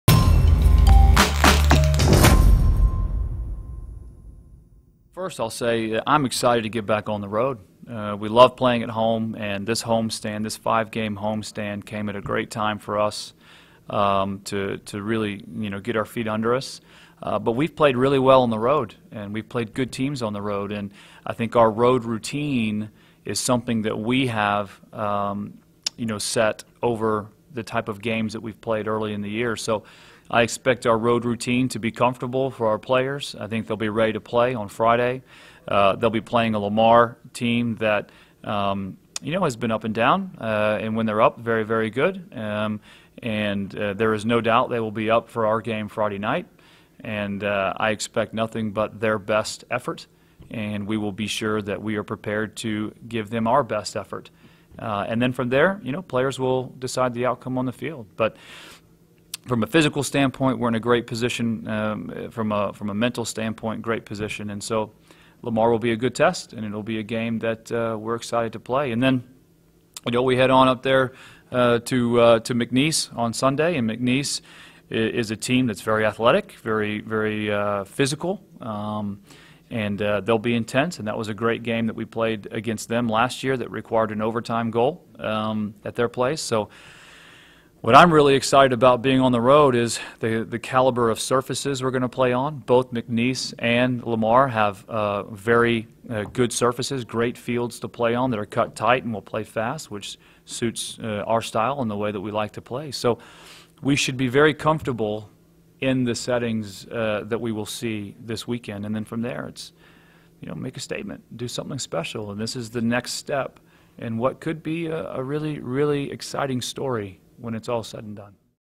Soccer | Lamar & McNeese Preview
Head coach Wally Crittenden previews SFA's upcoming road swing at Lamar and McNeese.